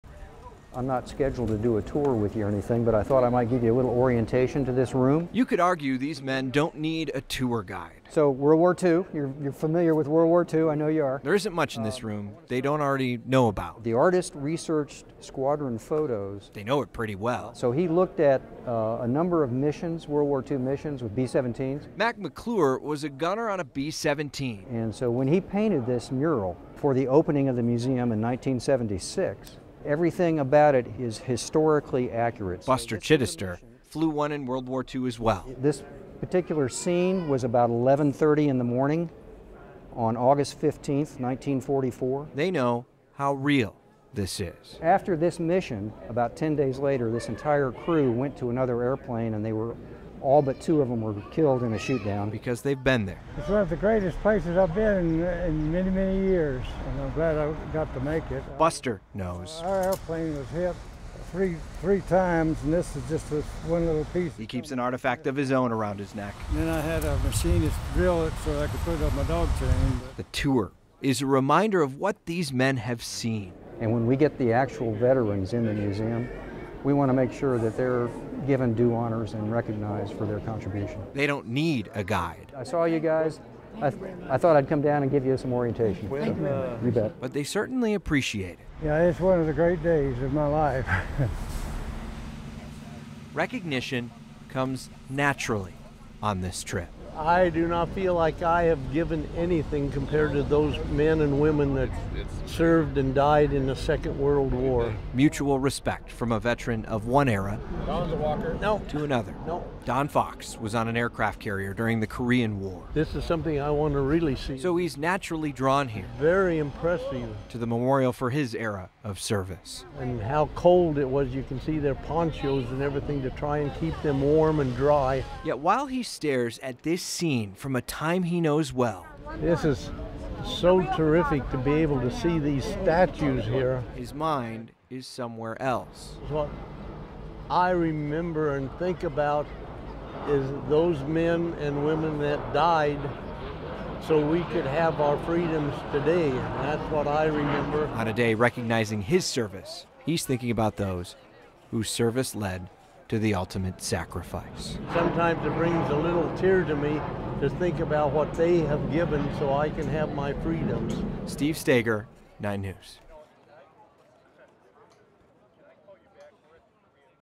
Honor Flight: Perspective and sacrifice of a Korean War veteran
A veteran of the Korean war reflects on his time in service, and the sacrifice of others, as he visits the Korean War Memorial.

A World War II pilot shares an artifact from when his plane was shot down. Both recall stories, sacrifice, and honor.

Donate to the 9NEWS Honor Flight telethon and make trips like this possible for World War II, Korean War, and Vietnam War veterans.